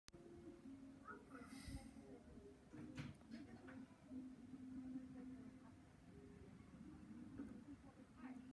What’s Your Opinion On This Movie: 102
Easily Right Now The Best Fast And Furious Movie And My 4th Favourite Movie Of 2015